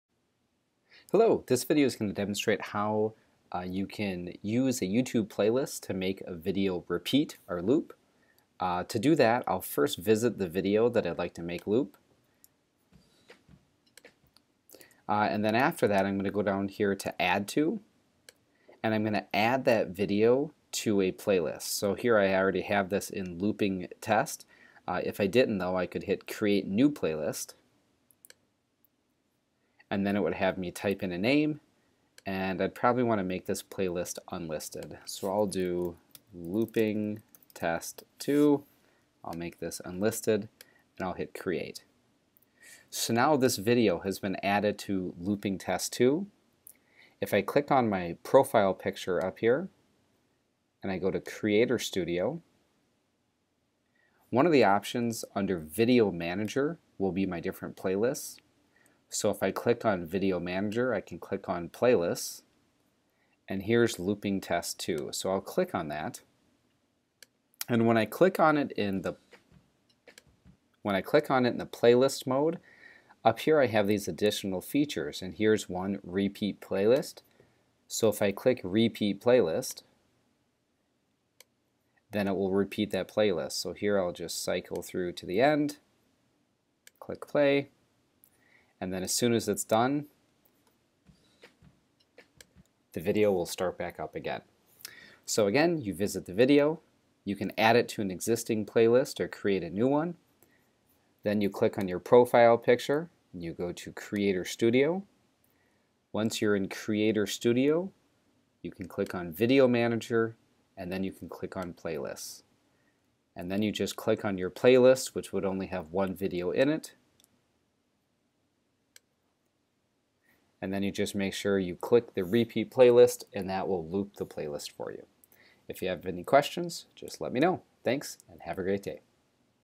Looping a YouTube Video Using a Playlist
This video demonstrates an easy way to loop a YouTube video using the repeat feature of a playlist.